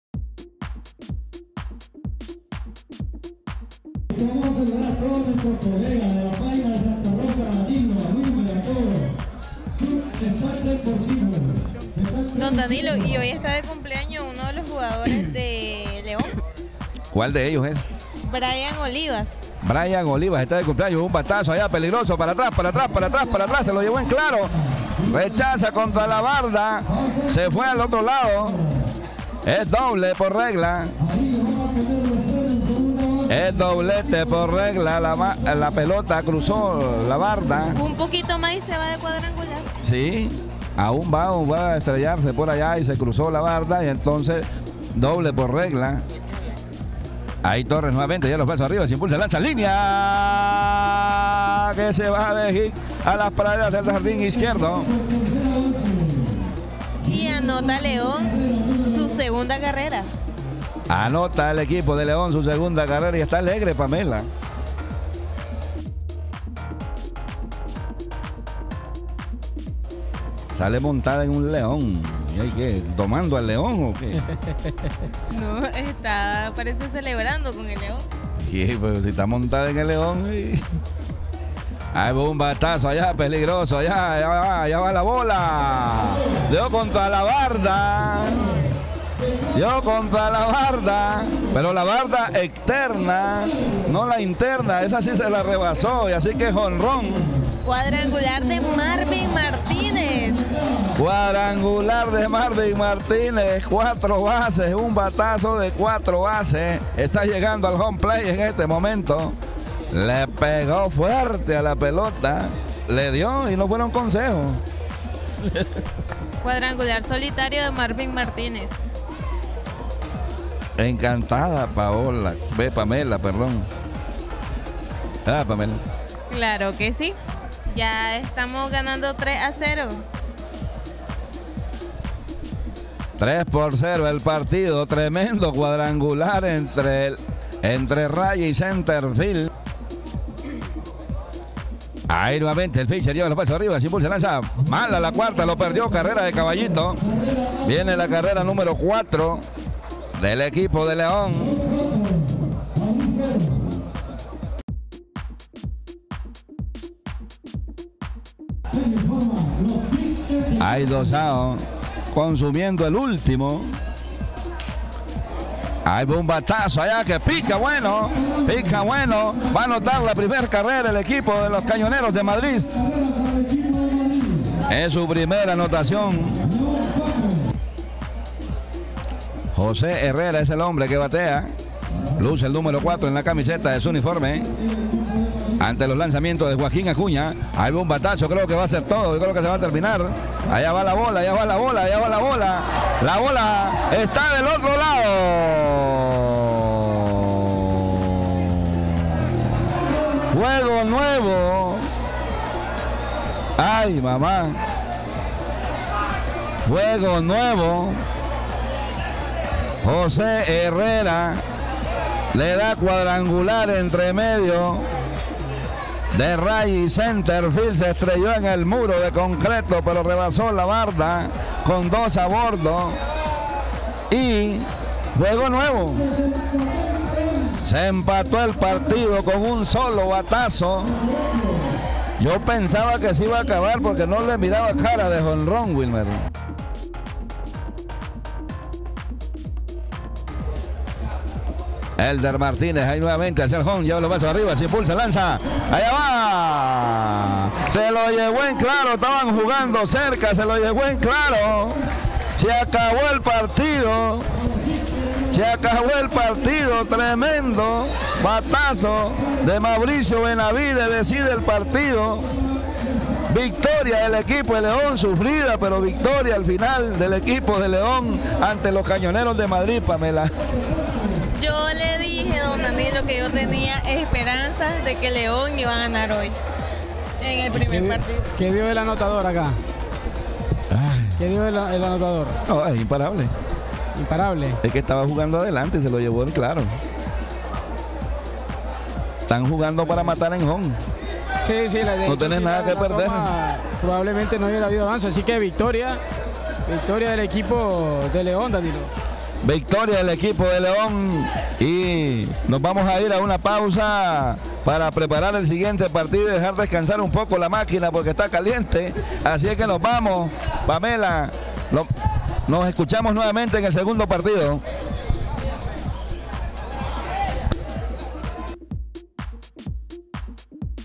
Resumen. Madriz vs León, Fecha 7, Juego 3
En un dramático final León se impuso en 9 entradas a Madriz en el tercer partido de la serie.

Madriz 4 - 5 León.
JG: Joaquín Acuña
JP: Elder Martínez

Estadio Juan Ramón Blancher, El Sauce